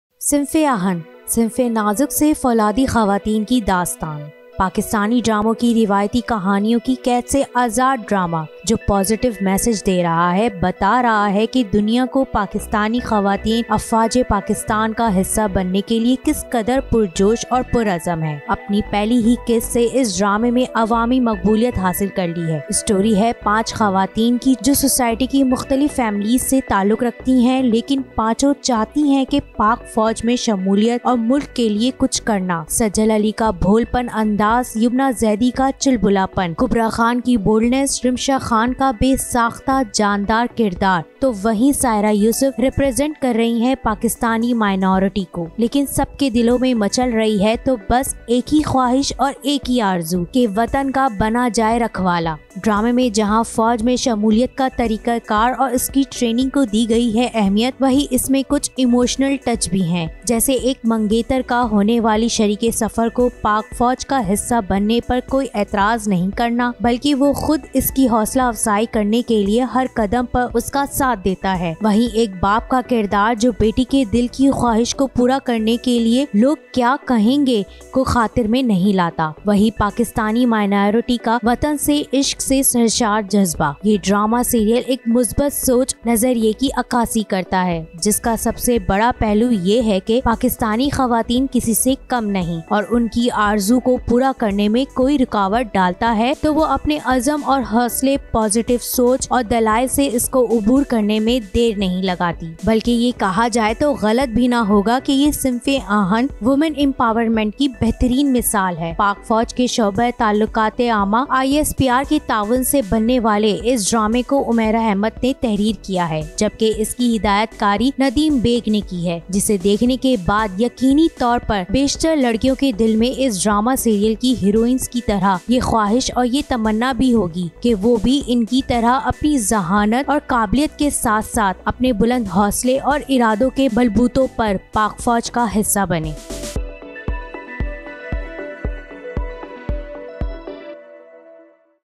Sinf e Aahan | Ep 01 To Ep 04 Reviews | Taar | ISPR |Sajal ali |Kubra khan | Ramsha khan|Syra yousuf
Sinf -e-Ahan, a  story revolving around five women who from various social backgrounds want to join Pakistan's Army while being supported from their family members. This play also discusses about the respectable minorities living in Pakistan and want to be the part of Pakistan's Army.